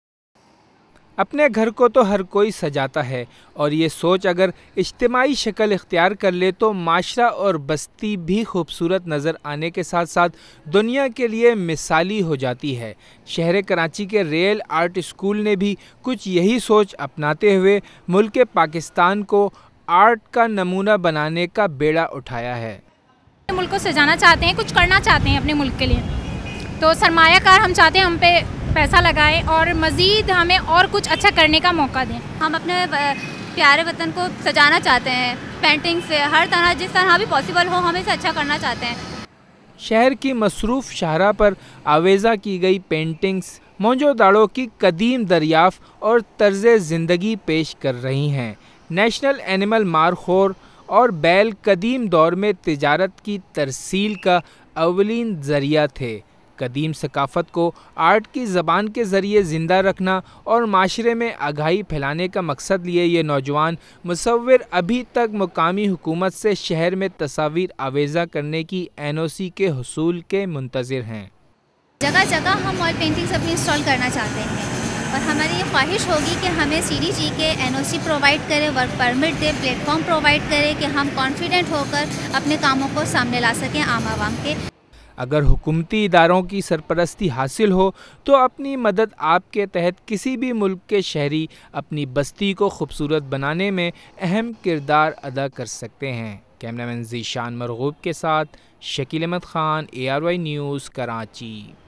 NEWS HEADLINES LIVE | Cultural Historical painting on Road Karachi | ShakeelReporter
NEWS HEADLINES LIVE | Cultural Historical painting on Road Karachi
Real Arts Organization plan to paint Cultural Historical painting on all Karachi Road wall
Mohinjo Daro and sindh Cultural Painting etc.